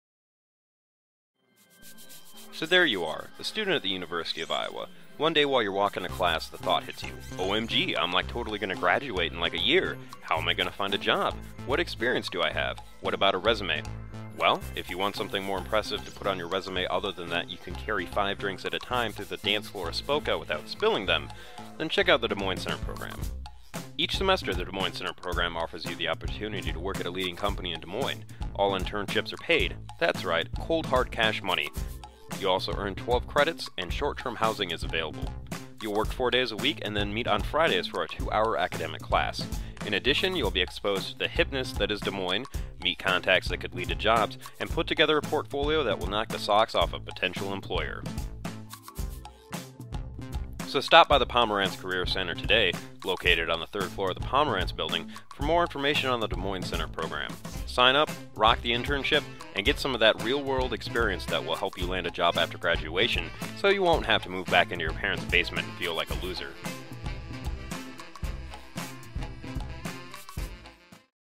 Des Moines Center Program
Promotional Video for the Pomerantz Career Center's Des Moines Program, which each semester offers University of Iowa students the opportunity to intern at leading companies in Des Moines while earning 12 credit hours.  Information about this program can be found

(Giving credit where credit is due: props to Ben "Yahtzee" Croshaw of Zero Punctuation fame for inspiring the style of this video.